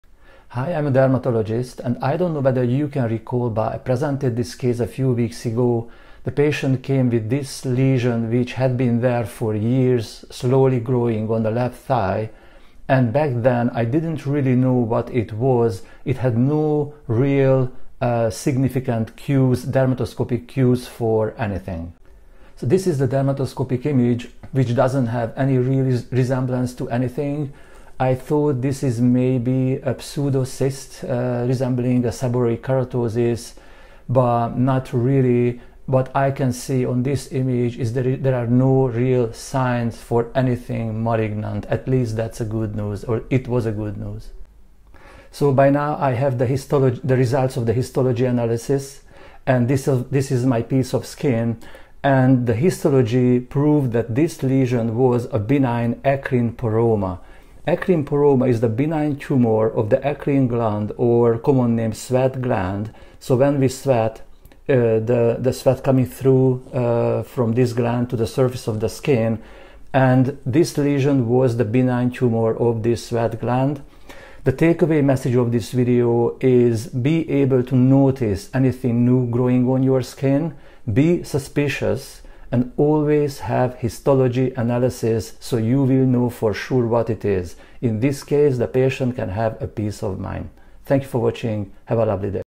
Tumour in the sweat gland? - A Dermatologist explains
As a dermatologist, I come across many unique cases, and this one was particularly intriguing. A patient presented with a slow-growing lesion on their thigh that had been there for years. At first, it was a challenge to identify—it had no clear dermatoscopic clues and didn’t fit the profile of common skin conditions like seborrhoeic keratosis.

After performing a histology analysis, we discovered it was an Eccrine poroma, a benign tumour of the sweat gland. Thankfully, no signs of malignancy were found, providing the patient with peace of mind.

💡 Key takeaway: If you notice something new or unusual on your skin, always seek a professional opinion and, when in doubt, confirm with histology. It’s the best way to get a clear diagnosis and stay on top of your skin health.